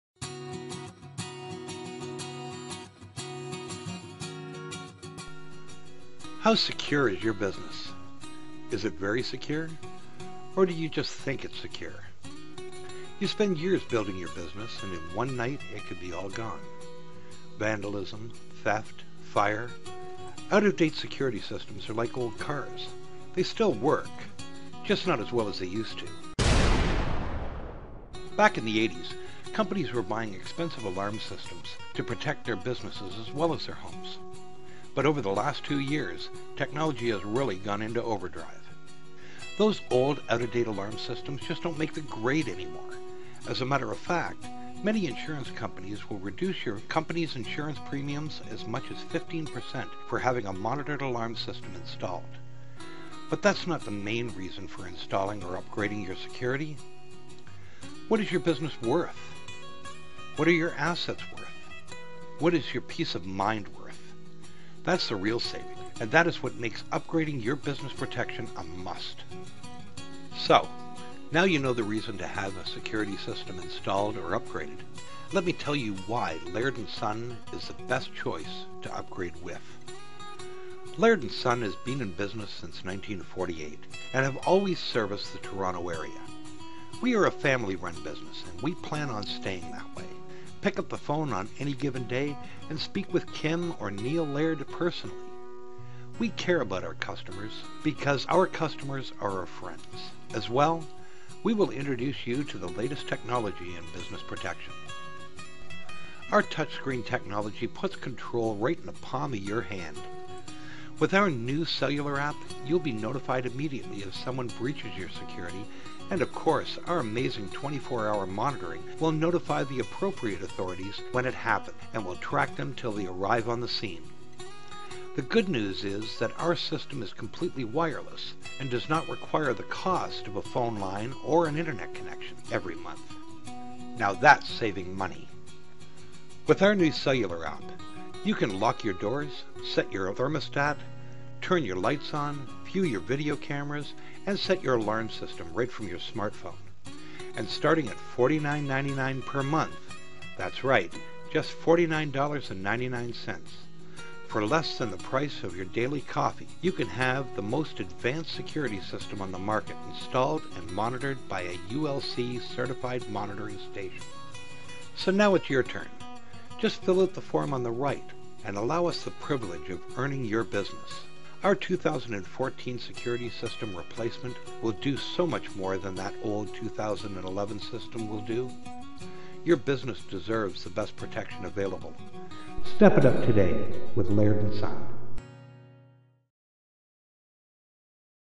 Laird & Son Intro
How Secure is your business?...Is it very secure?...or do you just think its secure? Laird & Son has been in business since 1948 and have always serviced the Toronto area. We are a family run business and we plan on staying that way. Our 2014 security system replacement will do so much more than that old 2011 system will do. Your business deserves the best protection available. Step it up today with Laird & Son.